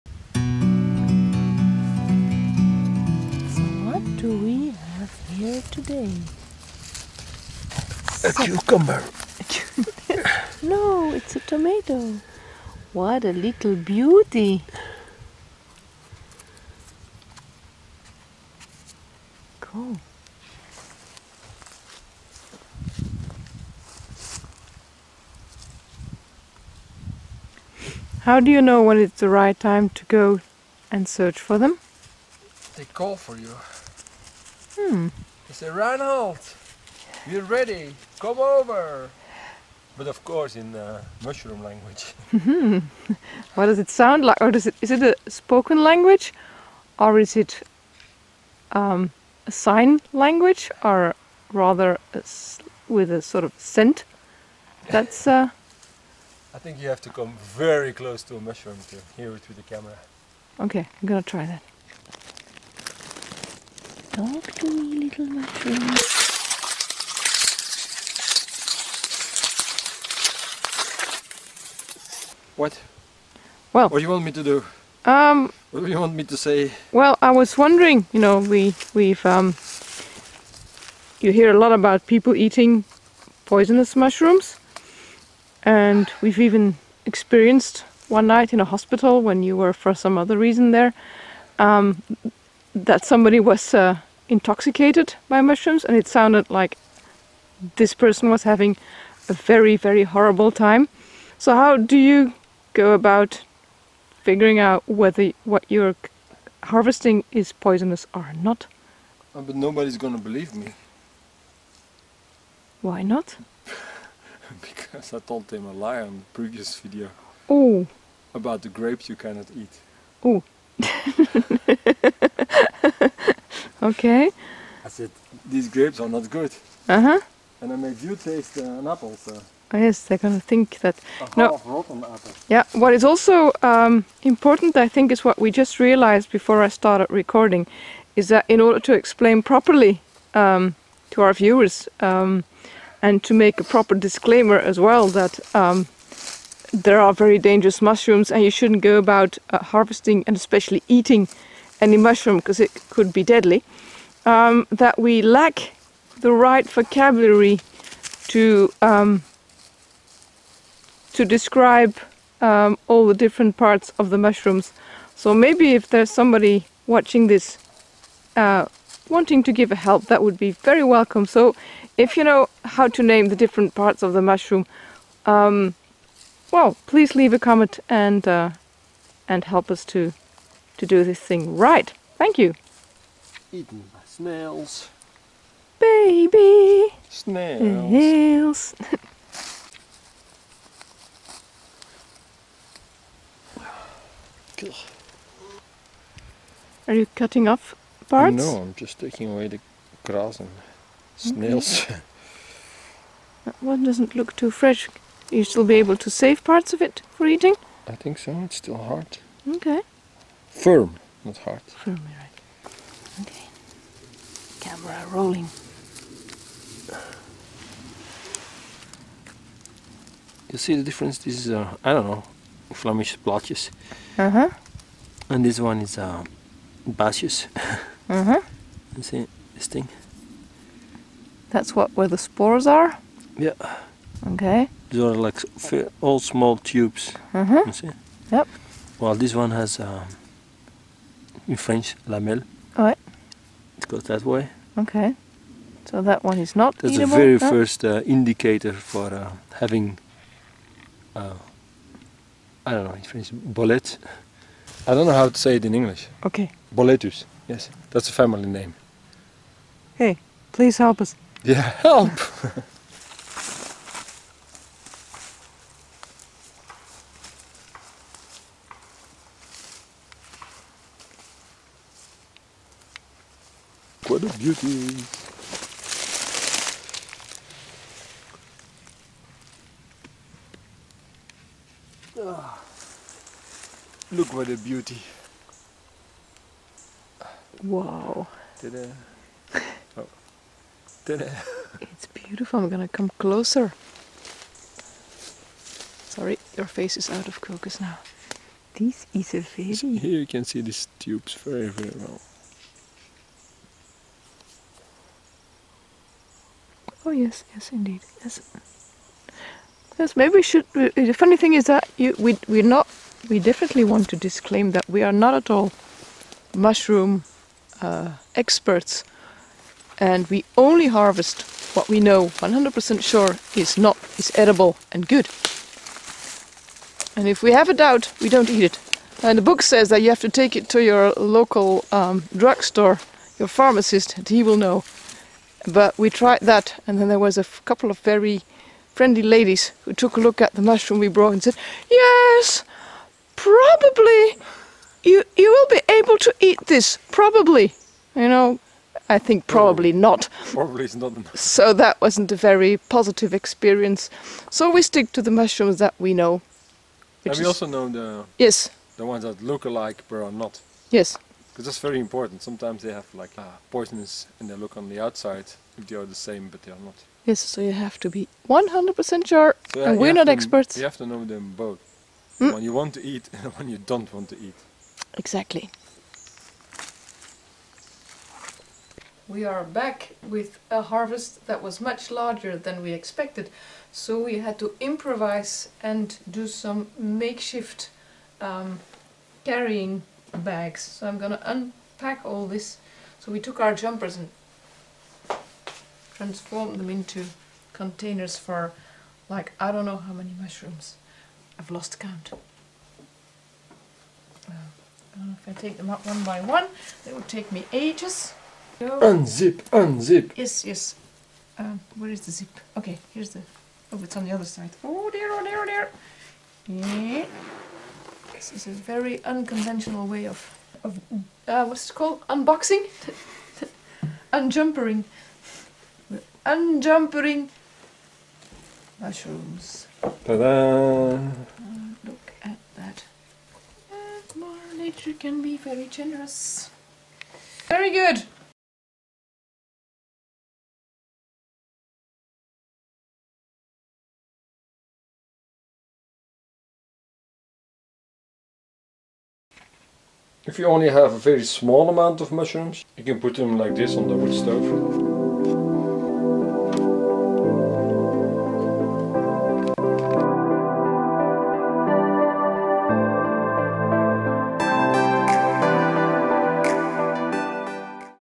Foraging in the woods we found beautiful gourmet mushroom 🍄🍄 : King bolete, cepe, penny bun
We went mushroom picking and  returned with several kilos of  edible 'boletus edulis' which we've dried slowly above the wood stove.